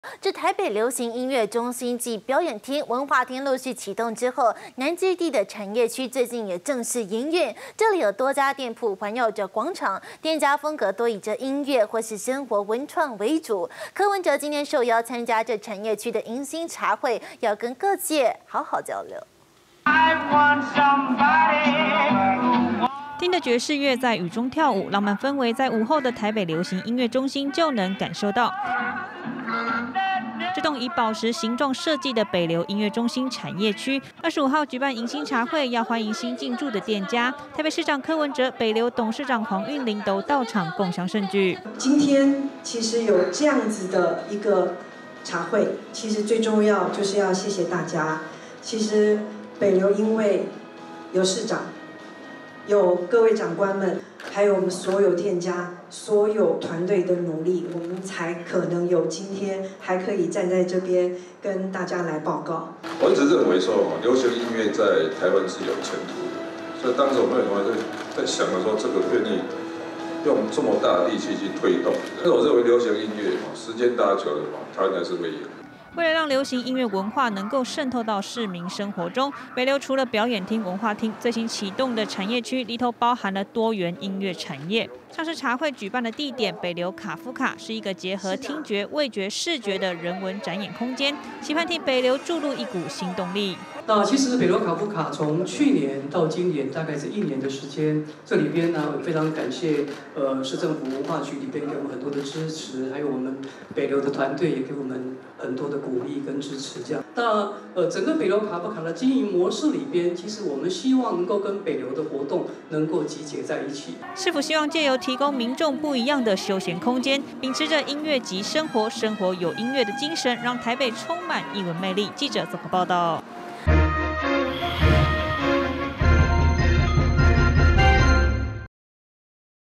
北流南基地產業區迎新茶會 柯文哲現身交流
台北流行音樂中心繼表演廳、文化廳陸續啟動後，南基地的產業區，近期也正式營運，這裡以多家店鋪環繞著廣場，店家風格多以音樂或生活文創為主，市長柯文哲25日受邀參與「產業區迎新茶會」，要與業界各代表好好交流。